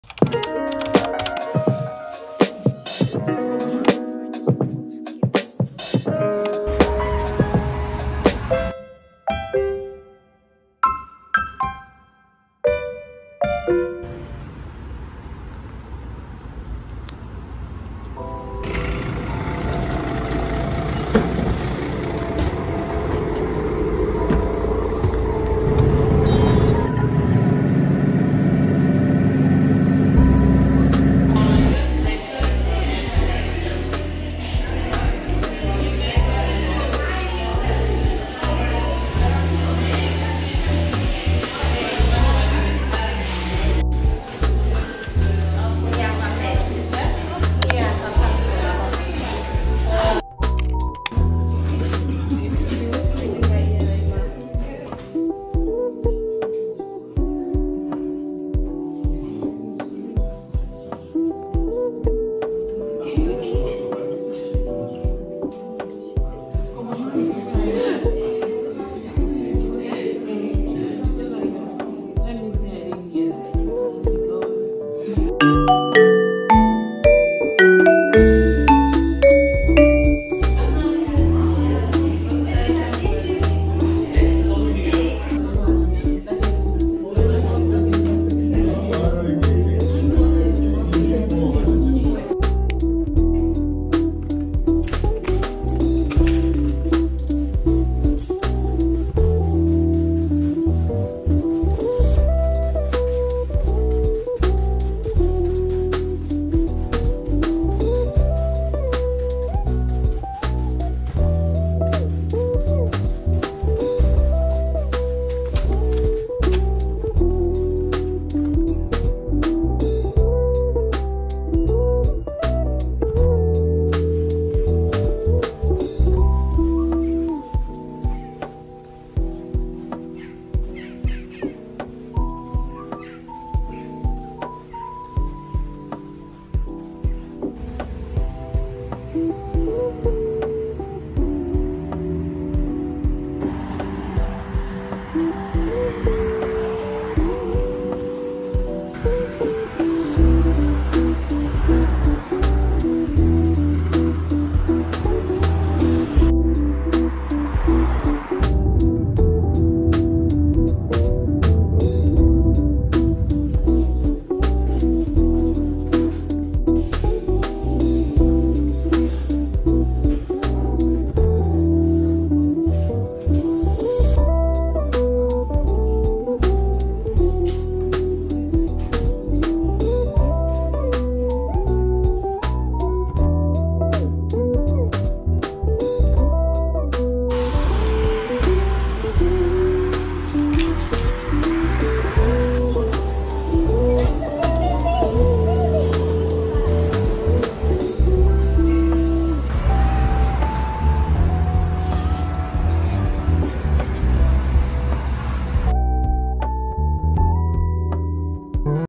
Last 2 Days of University in Poland 🇵🇱 | اخيرا ساليت قراية✨ أجي دوزو معيا يومين في الجامعة ببولندا
Come along for my last 2 days as an international student in Poland! 🇵🇱 From finishing up university to making the most of my final days in the city, see how I spent my last moments.